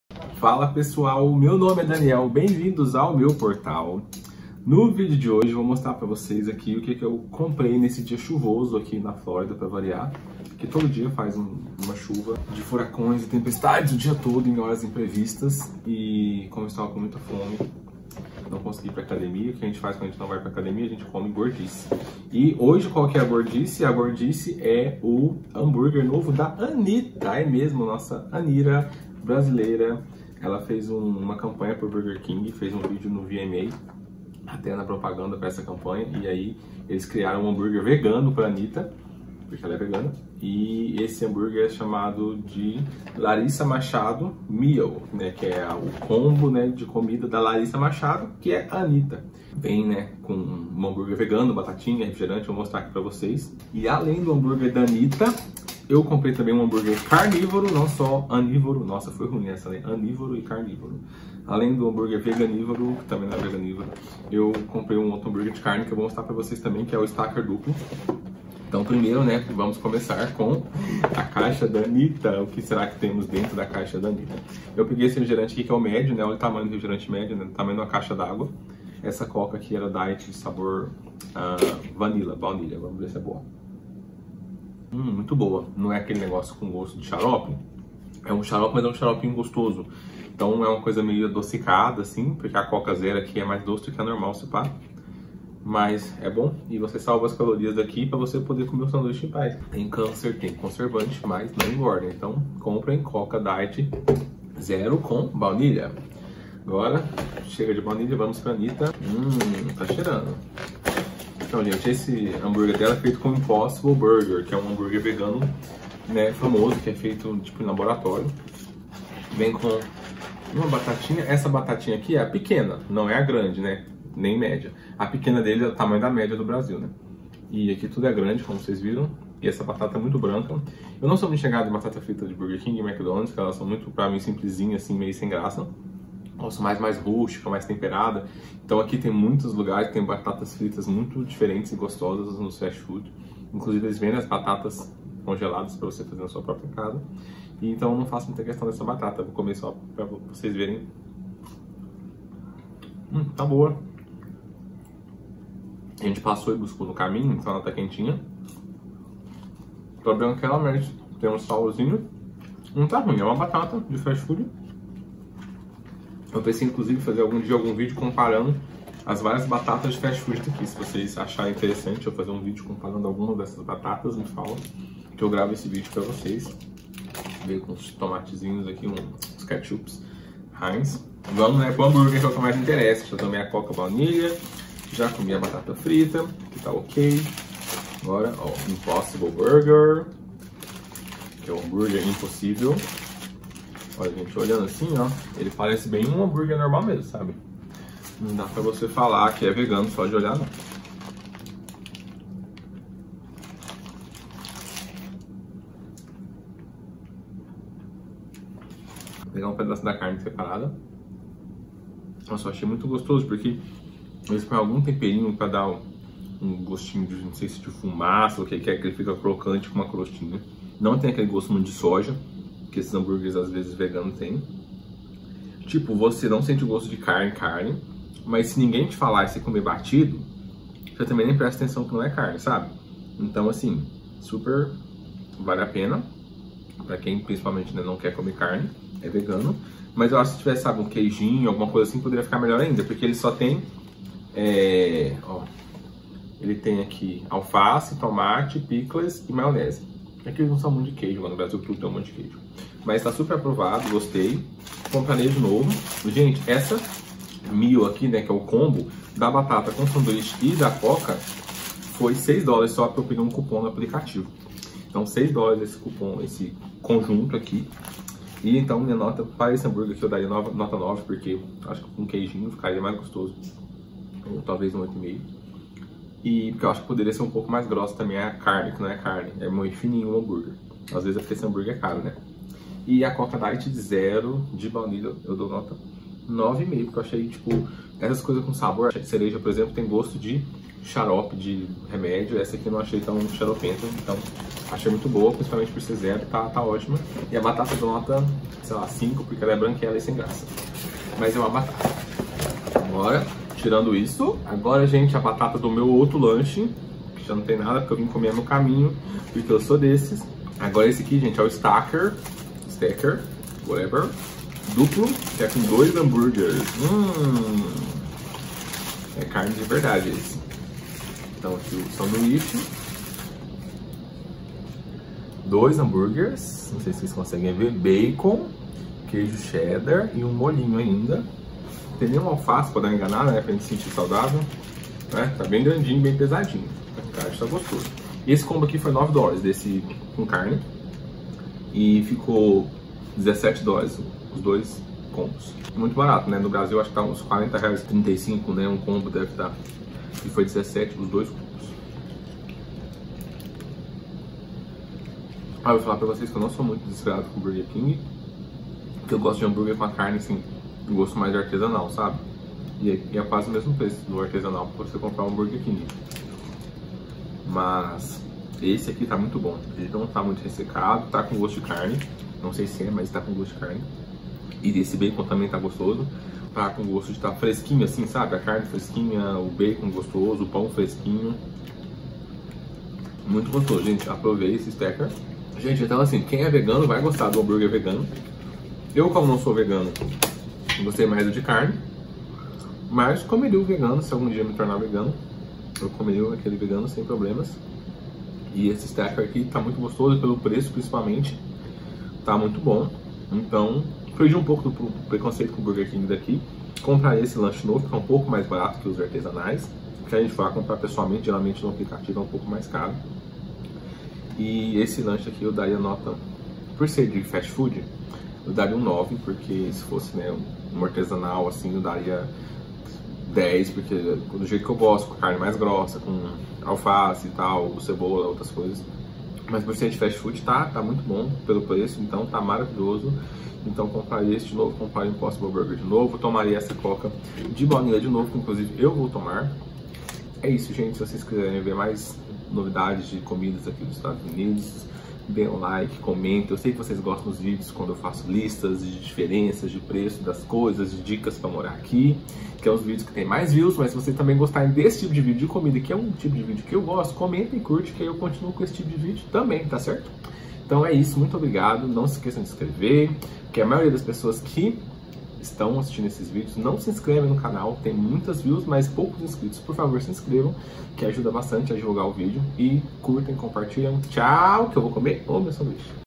PROVANDO O BURGER KING DA ANITTA - IMPOSSIBLE BURGER LARISSA MACHADO
Nesse vídeo experimentei o hamburguer vegano da Anitta, o Larissa Machado meal, que vem também com batata e refrigerante (provei a coca diet vanilla).
Além disso, para satisfazer meu lado carnívoro, comprei o combo do double stacker, qual será que gostei mais???